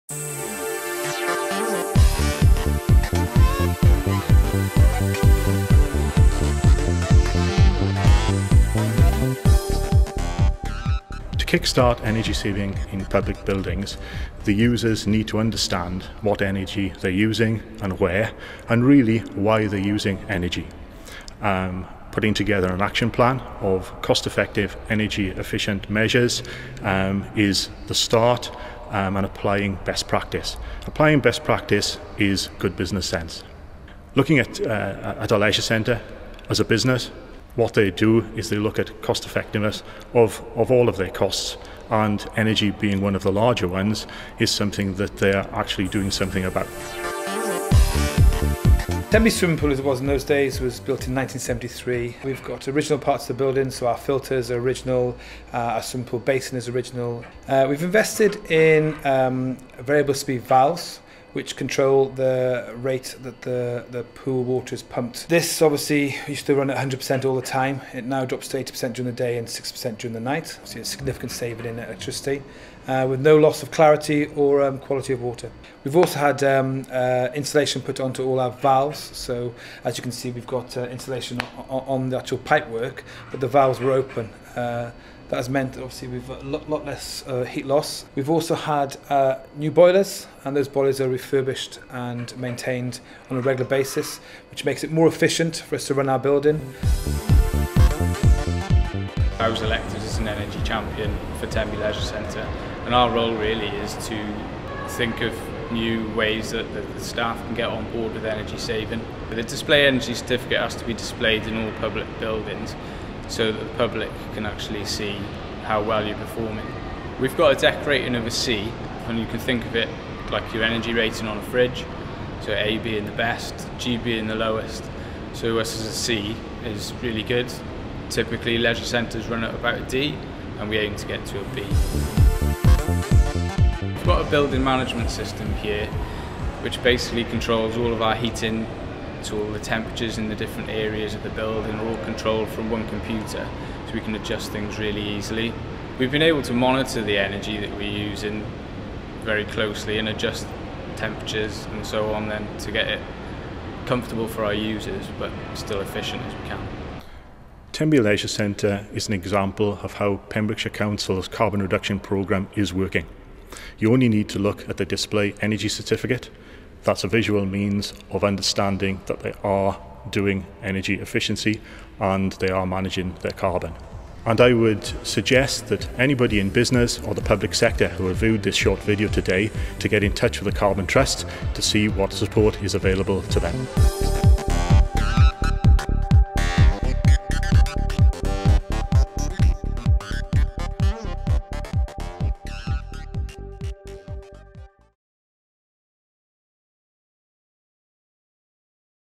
Championing energy efficiency at Tenby Leisure Centre
For more information on the Carbon Trust's work with Welsh companies and organisations